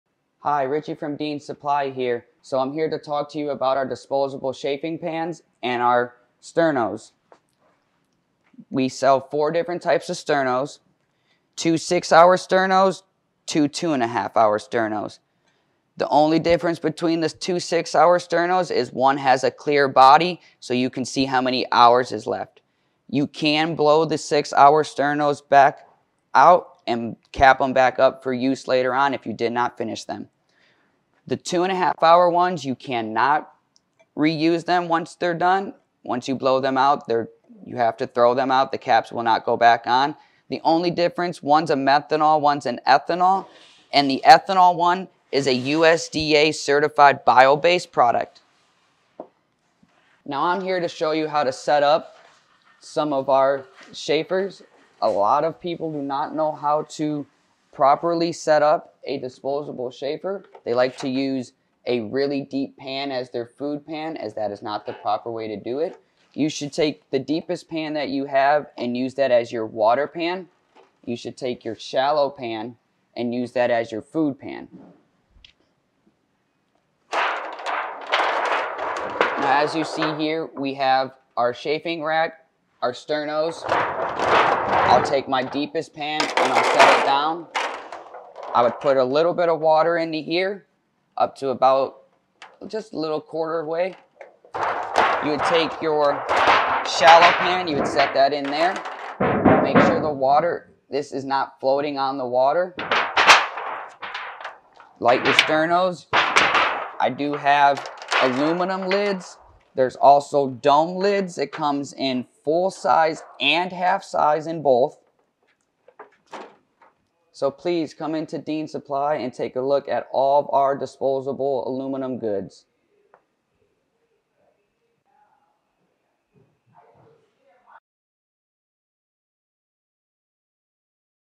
Proper way to use a disposable food pan with water bath.
Something commonly used but often misunderstood is how to properly use a disposable chafing pan to heat an aluminum food tray. There's one key piece you're probably missing. Watch and learn!